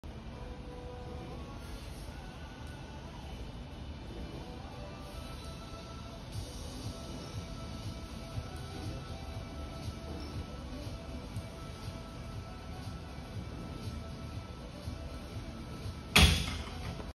Tall Kneeling Rotation
Both knees on the floor
Both sets of toes engaged into the floor.
Keep hips and belly bottom square while shoulders and t-spine rotate